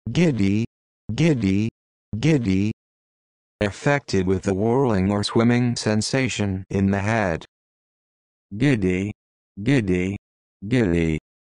SAT Vocabulary Words and Definitions — Giddy
This video shows you how to say or pronounce Giddy and gives the definition of Giddy.

What's your definition of Giddy?